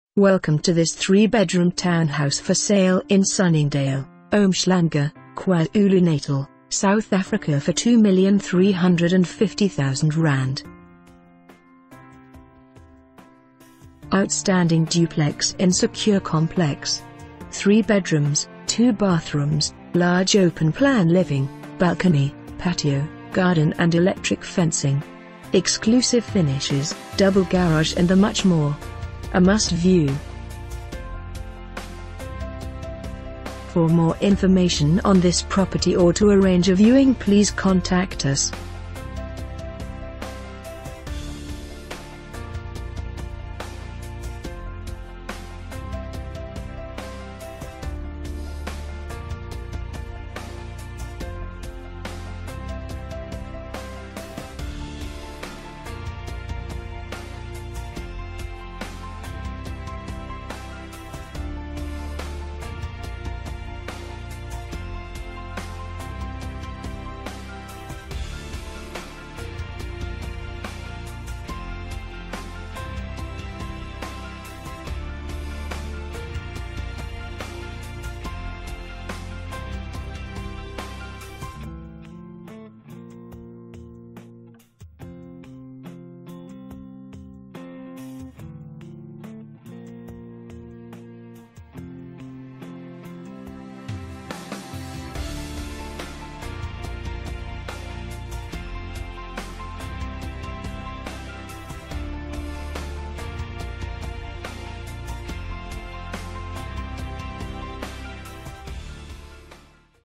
3 bedroom Townhouse For Sale in Sunningdale, Umhlanga, KwaZulu Natal for ZAR 2,350,000
OUTSTANDING DUPLEX IN SECURE COMPLEX
3 Bedrooms, 2 bathrooms, large open plan living, balcony, patio, garden and electric fencing. Exclusive finishes, double garage and much more. A must view!